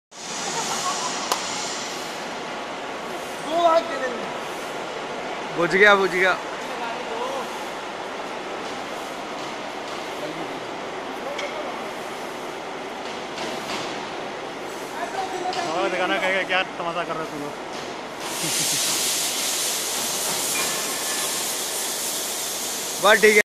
Prototype- floating hydro power generator
turbine at display is a prototype of a turbine installed on dam at pathankot, it is a very first concept in india for the floating type turbine produced by emerging energies pvt ltd. Mohali